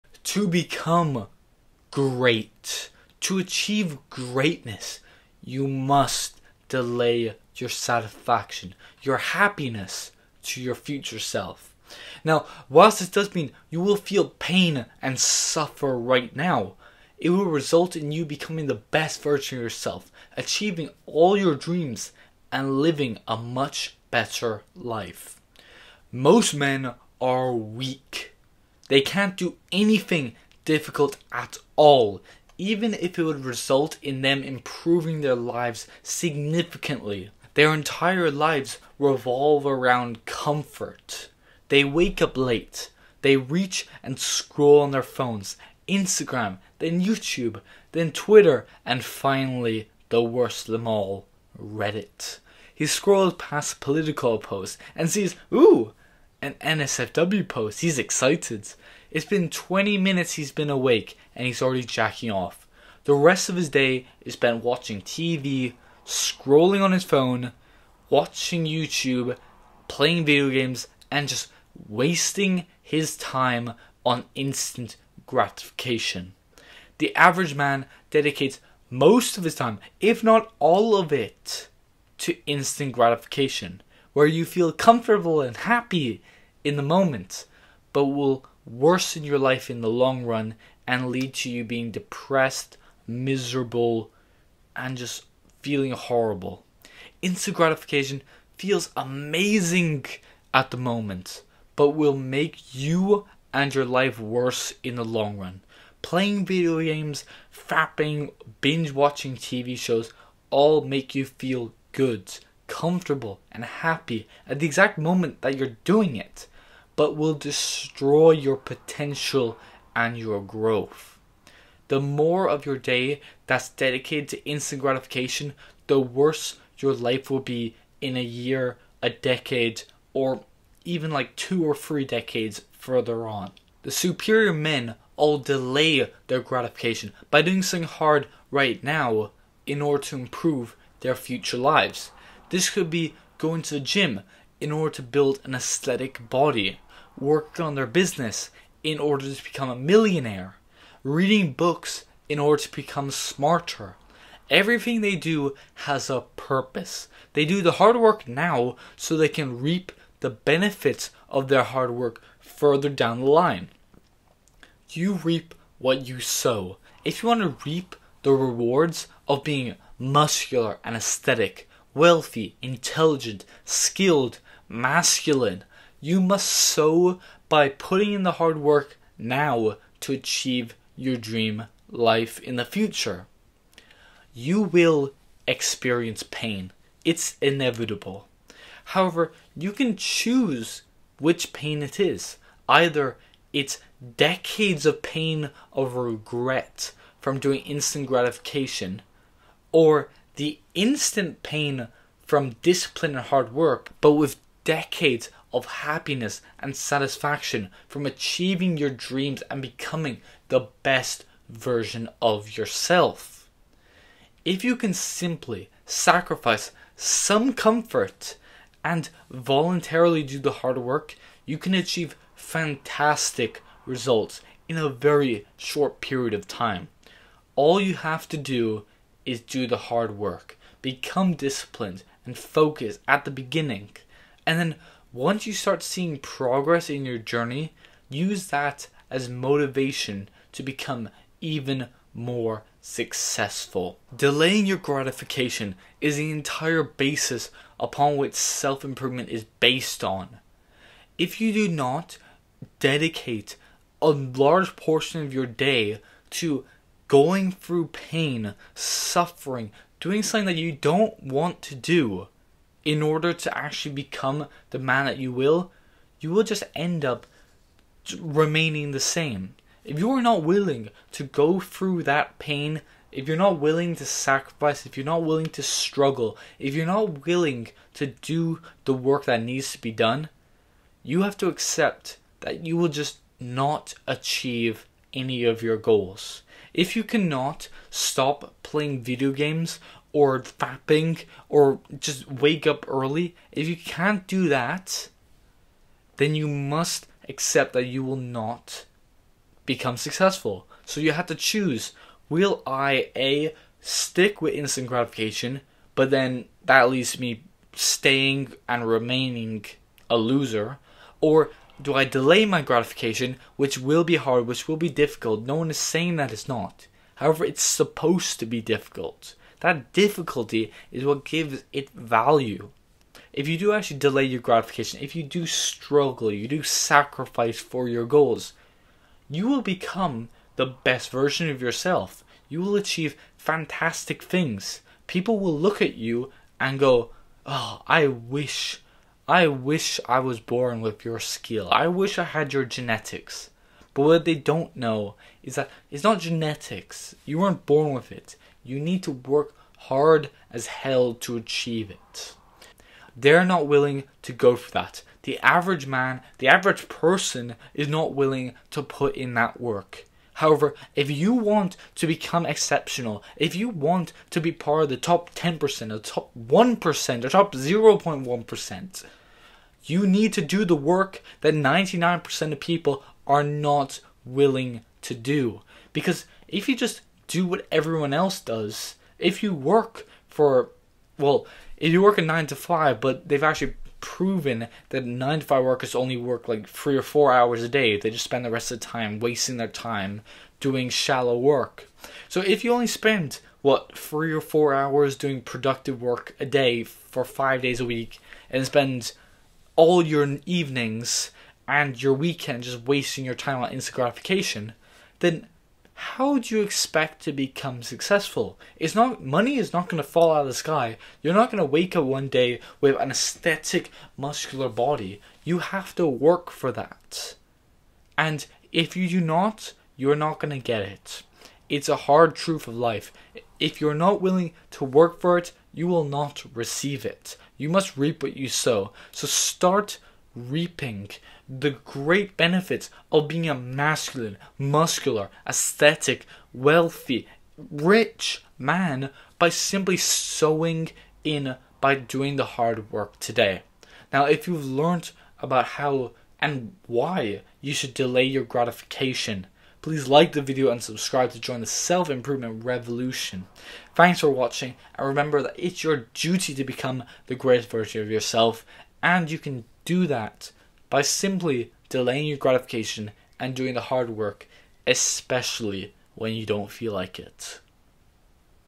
Delayed Gratification Will 10X Improve Your Life
To become Great, to achieve greatness, you must delay your satisfaction, your happiness, to your future self. Whilst this means that you will feel pain and suffer right now, it will result in you becoming the best version of yourself, achieving your dreams and living a much better life.

Delayed gratification is the ability to resist the temptation of an immediate reward in order to receive a greater reward in the future.